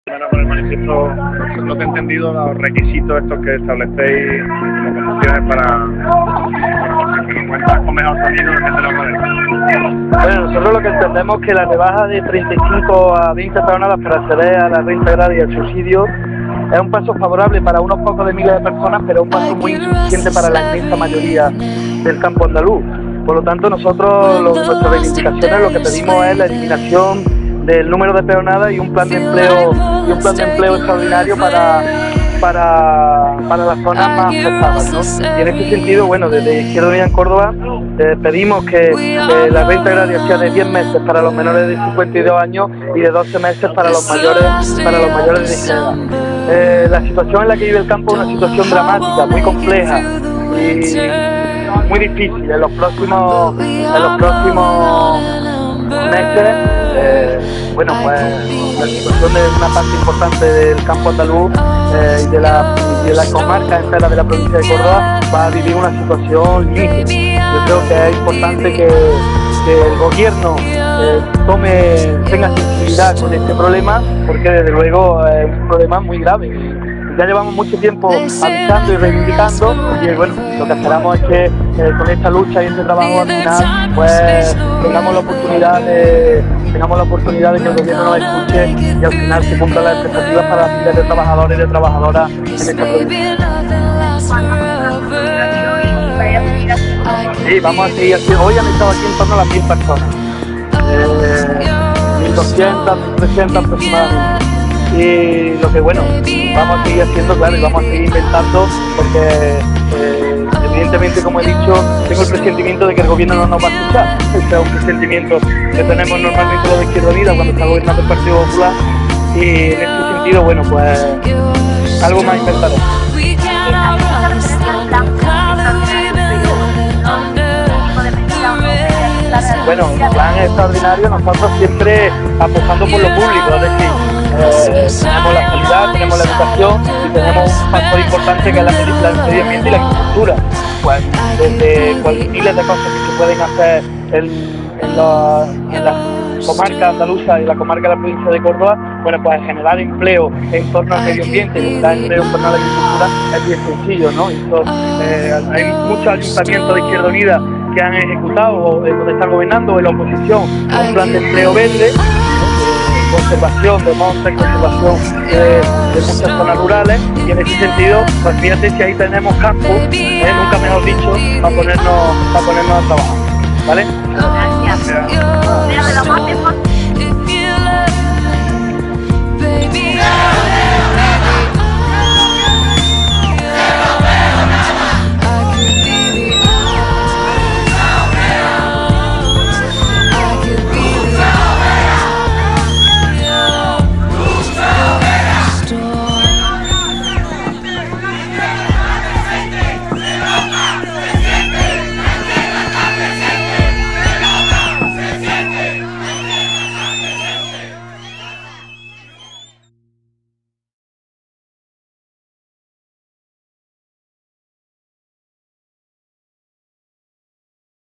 VIDEO MARCHA PEONADAS
MARCHA IU CÓRDOBA POR LA ELIMINACIÓN DE LAS PEONADAS Y LA PUESTA EN MARCHA DE UN PLAN DE EMPLEO EXTRAORDINARIO